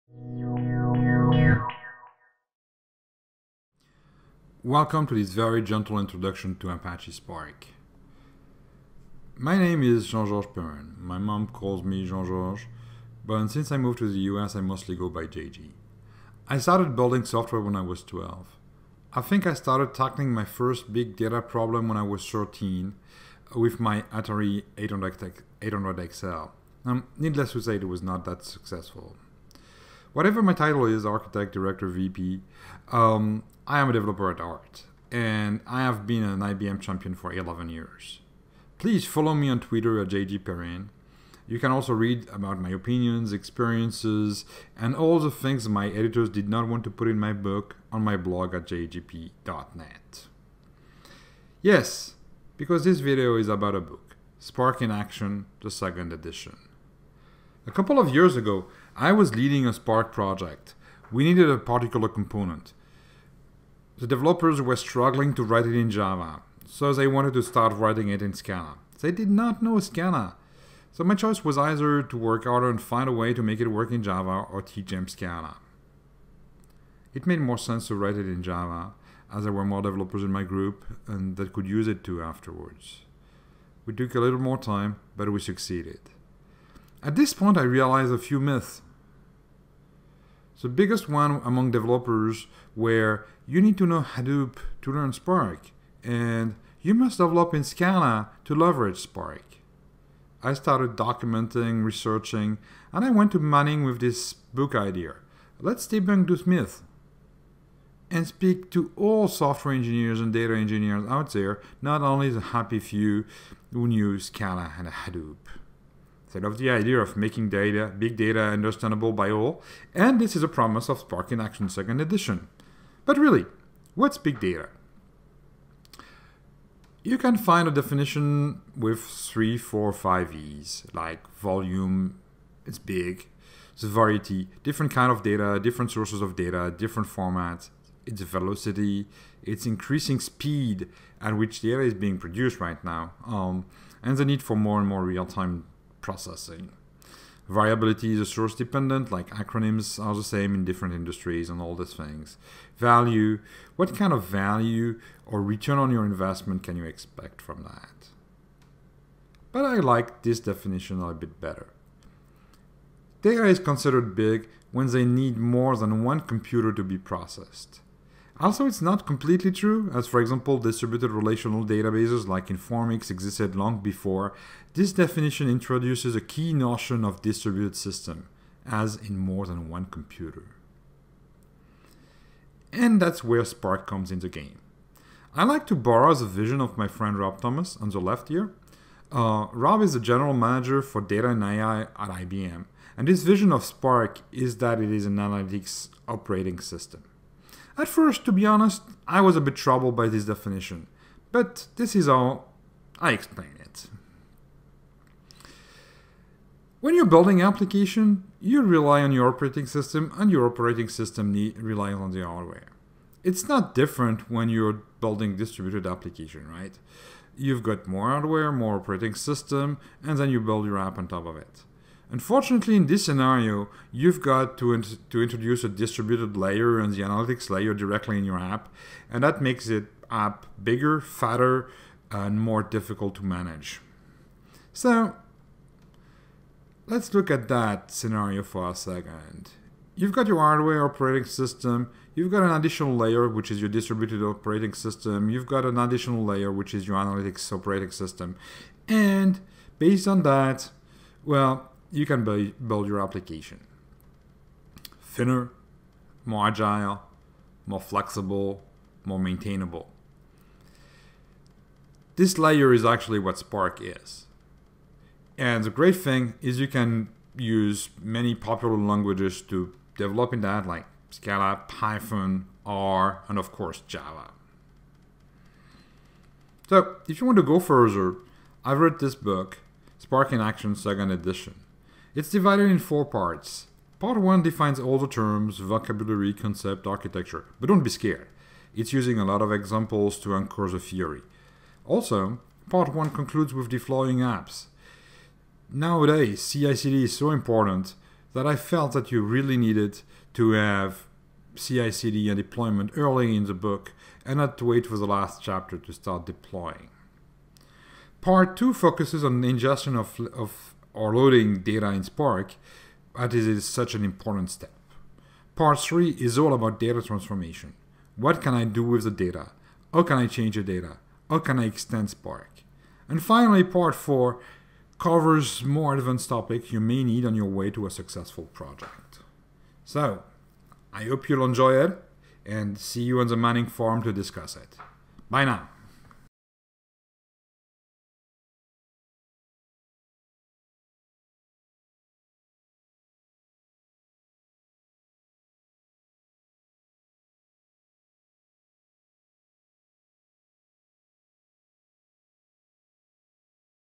Manning Introduces - Spark in Action, Second Edition
Author Jean-Georges Perrin introduces his book Spark in Action, Second Edition

The Spark distributed data processing platform provides an easy-to-implement tool for ingesting, streaming, and processing data from any source. In Spark in Action, Second Edition, you’ll learn to take advantage of Spark’s core features and incredible processing speed, with applications including real-time computation, delayed evaluation, and machine learning. Spark skills are a hot commodity in enterprises worldwide, and with Spark’s powerful and flexible Java APIs, you can reap all the benefits without first learning Scala or Hadoop.